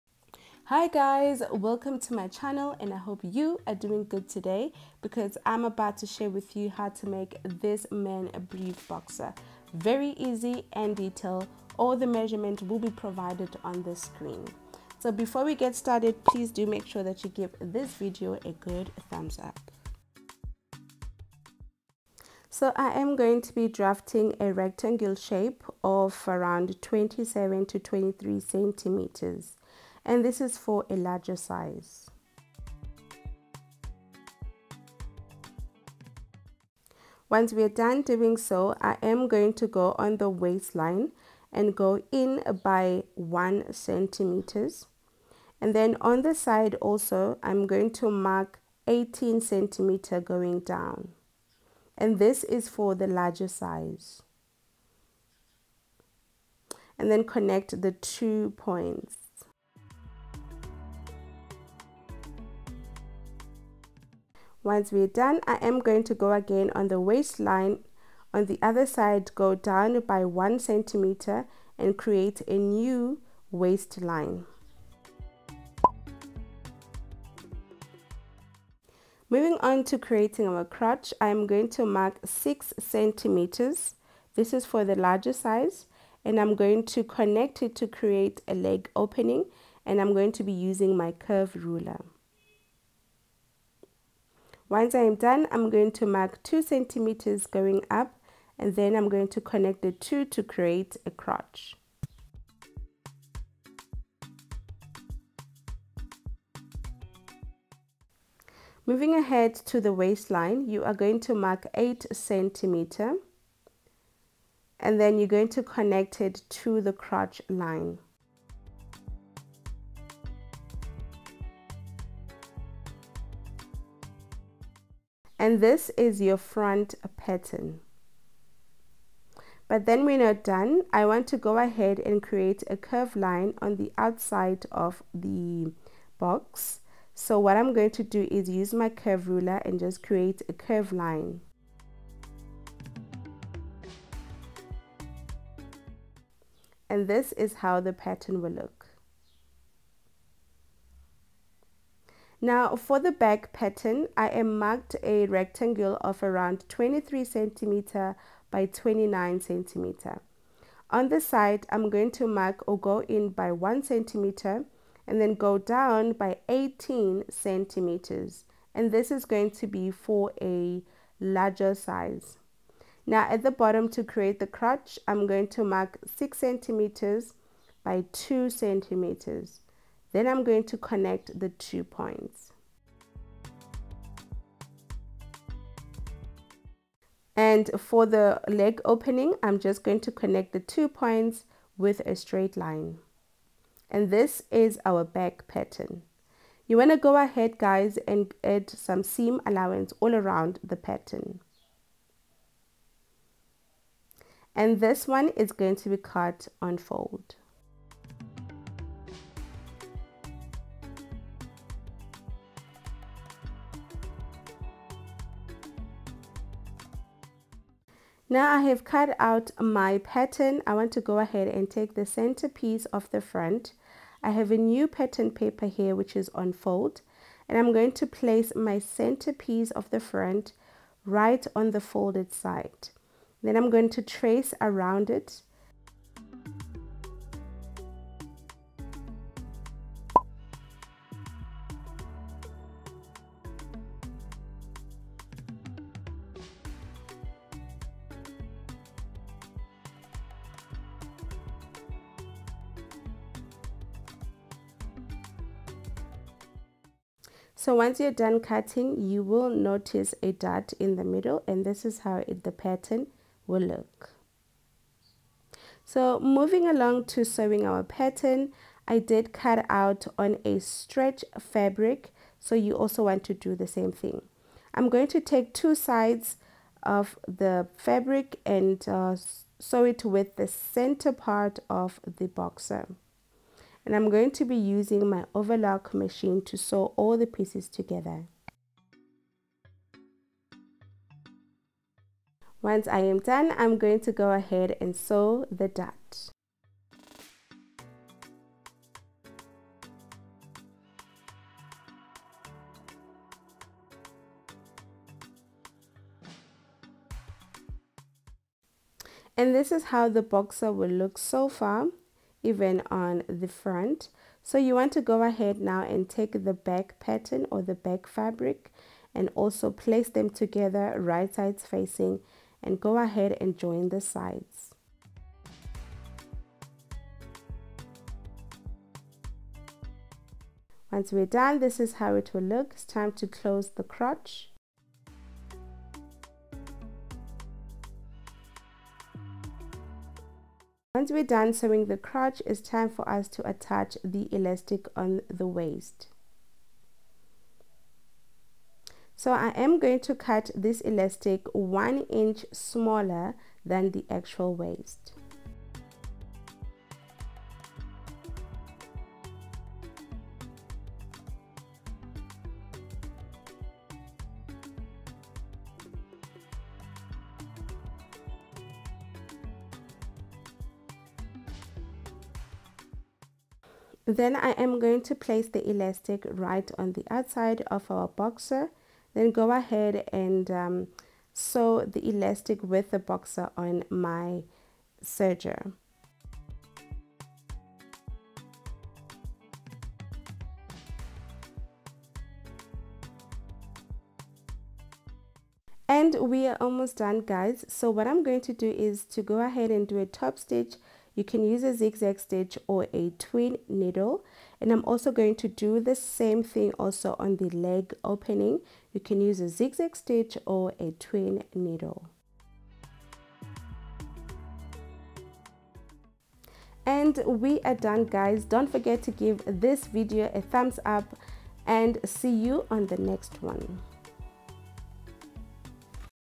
DIY MEN Brief Boxer.
cut an d stich brief boxer for men  from size small to xl measurements are provided.

00:00 -04:19 drafting pattern.
04:26-07:06 sewing pattern.

musician:jef.